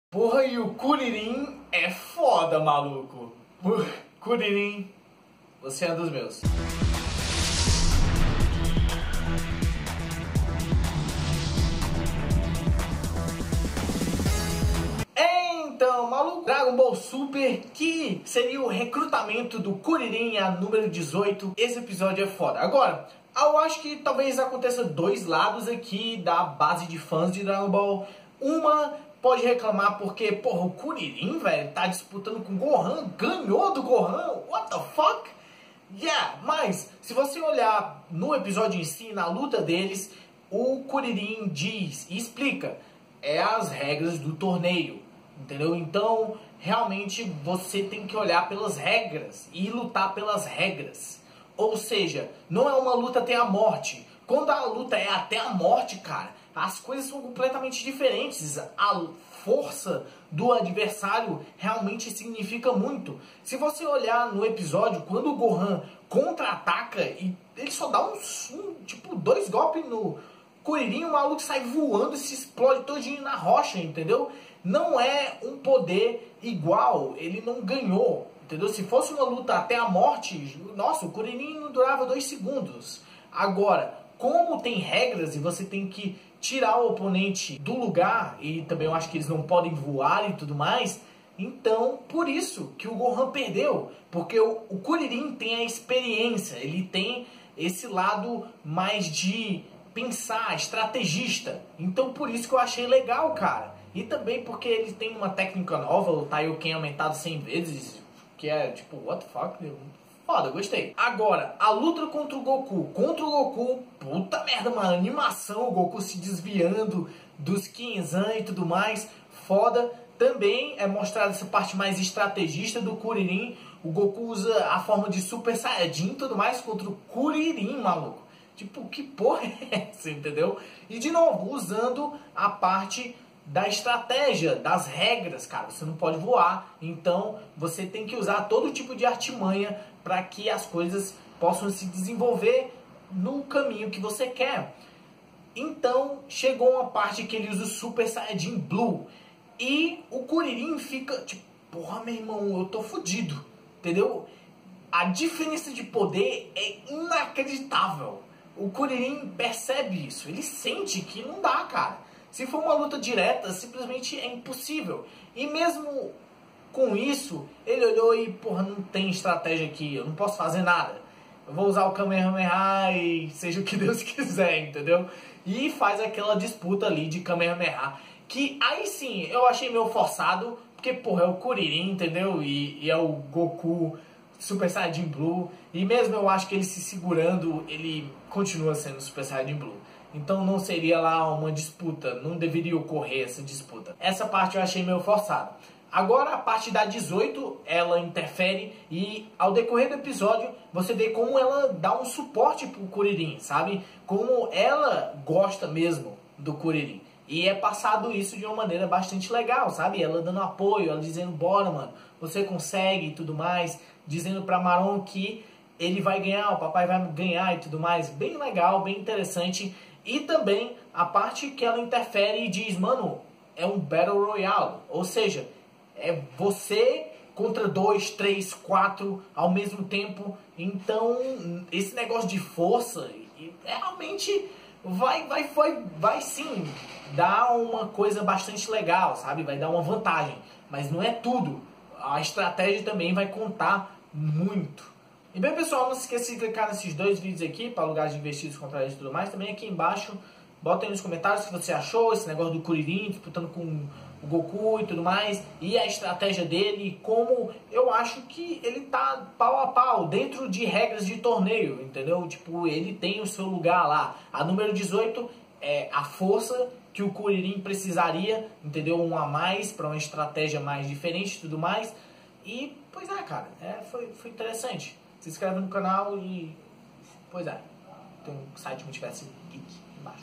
Dragon Ball Super Episódio 84 | Crítica
Dragon Ball Super Episódio 84 comentado onde temos kuririn vs goku  e recrutamento da Numero 18 e tambem vão atraz do Numero 17 para lutar no torneio do poder.